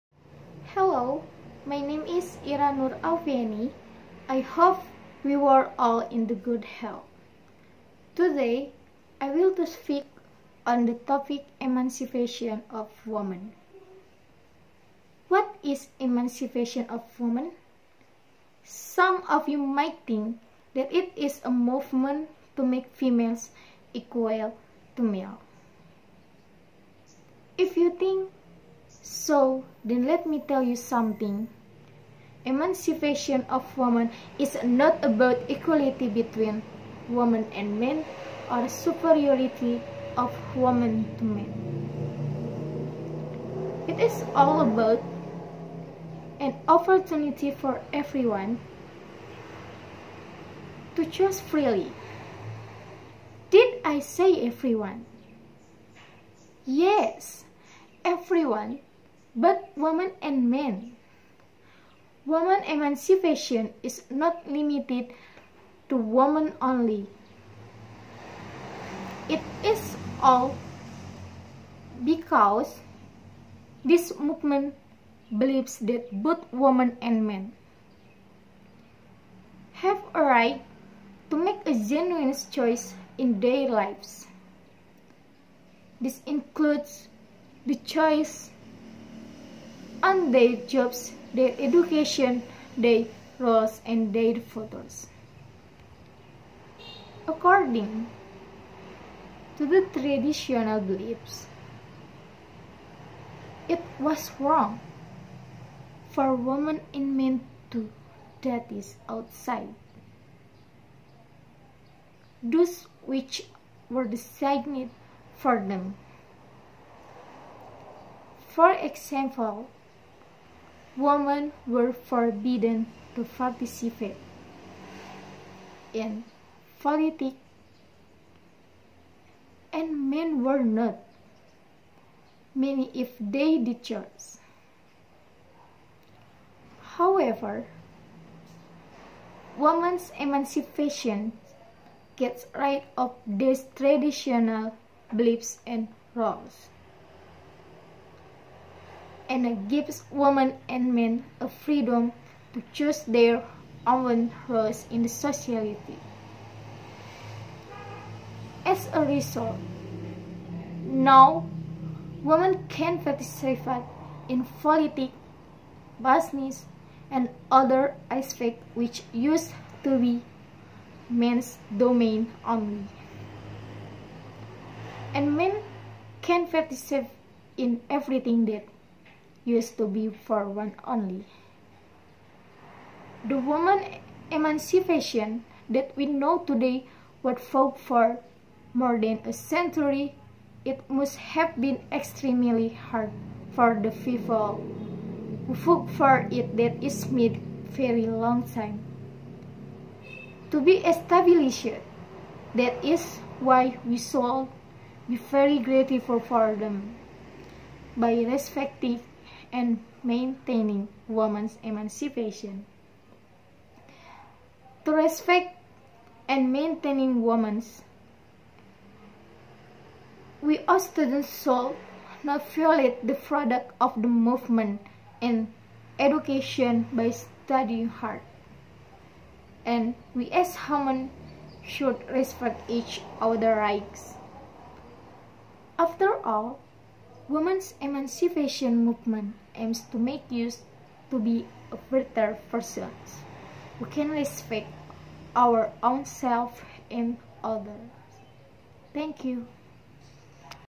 ENGLISH SPEECH - Emancipation Of Women |IRA NUR ALFIANI
Assignment of the English Language Final Exam - UAS B.Inggris
Name: Ira Nur Alfiani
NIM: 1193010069
Majors/Class/Semes: Familly law (Akhwal Syakhsiyah)/B/2
Lecturer: Nur Rochmat, S.S., M.Hum